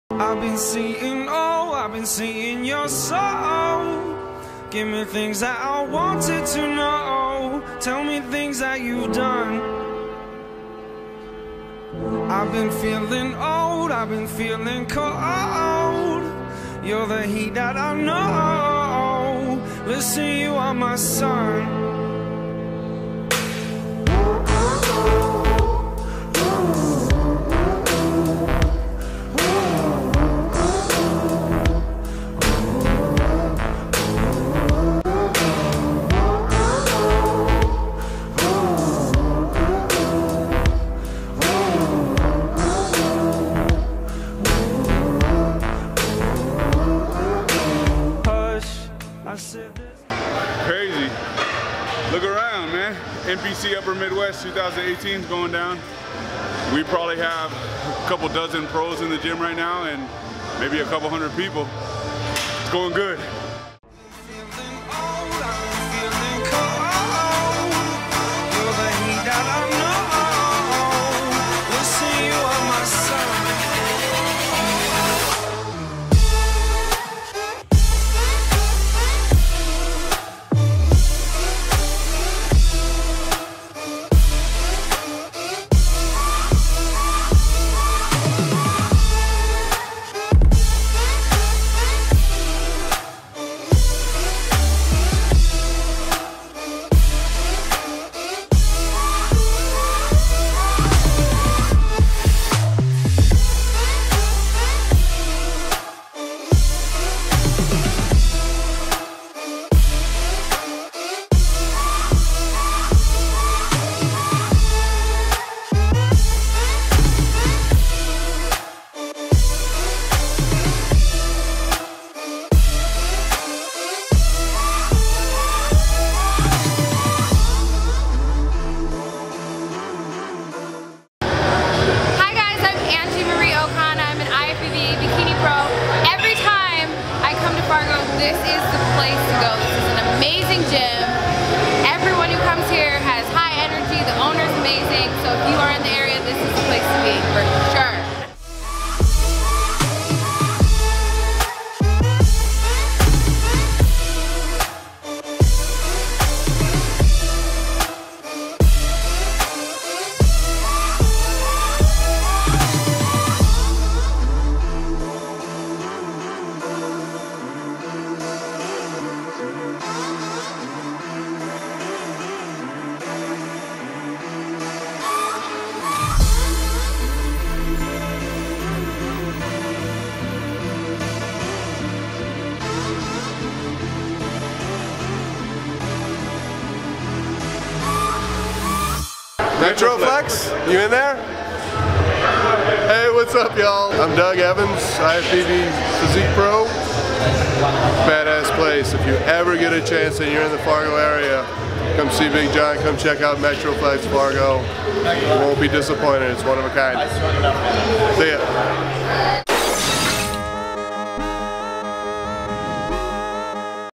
2018 NPC Upper Midwest weekend at Metroflex Gym-Fargo
Metroflex Gym-Fargo video edit from over the NPC Upper Midwest weekend, 2018!